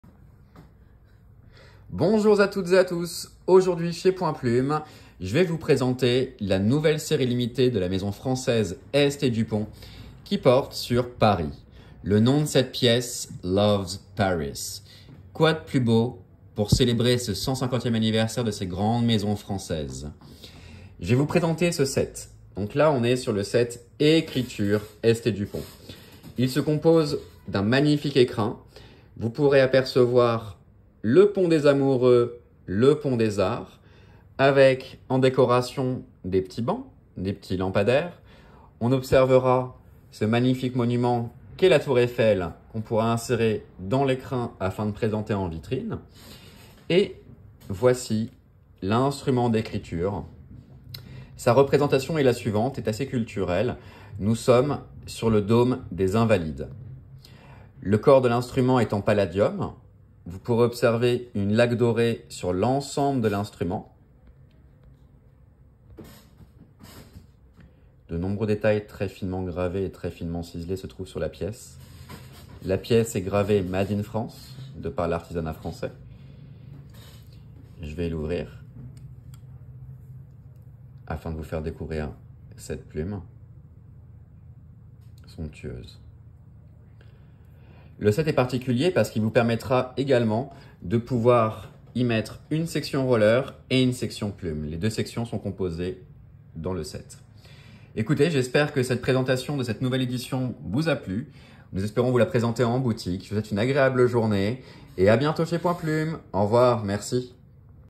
S.T.DUPONT : Set Ecriture “Loves Paris” Limited Edition 250 pieces
Inspirée par les monuments parisiens les plus emblématiques, L’Edition Limitée « S.T. Dupont Loves Paris » est un chef d’œuvre qui rend hommage au fondateur parisien de la maison, Simon Tissot Dupont, grand amoureux de Paris. Célébration du luxe ultime et du haut-artisanat, « S.T. Dupont loves Paris » synthétise à la fois l’héritage de la tradition, via la créativité sans limite de ses artisans, et les promesses de la modernité, par son design.

Confectionné avec des matériaux précieux, une base en bois laqué, une laque brillante et des finitions palladium, le stylo multifonction (section roller et section plume) S.T. Dupont Loves Paris rend hommage à un savoir-faire iconique de la maison : la laque placée. L’instrument d’écriture est proposé dans un écrin avec une plume en or 14 carats et un roller. Le coffret S.T. Dupont Loves Paris est une édition limitée et numérotée à 250 pièces.